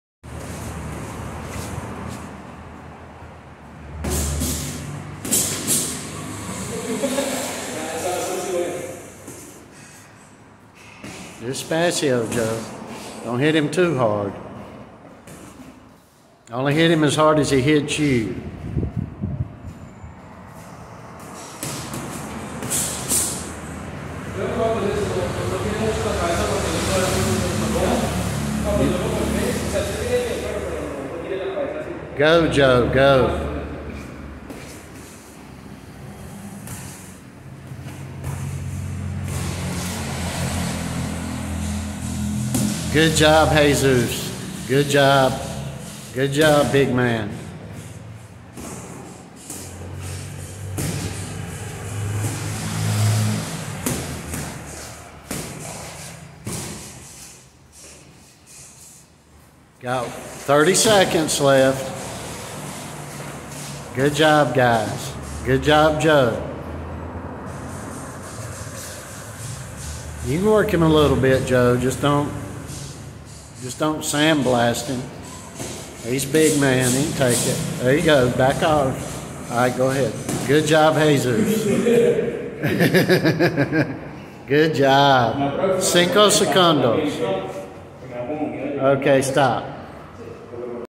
Sparring-14 Y.O. learning to box big men I'll eventually face in the ring.
Here I'm at 178 lbs sparring a 6'4 1/2" big 230 pound man.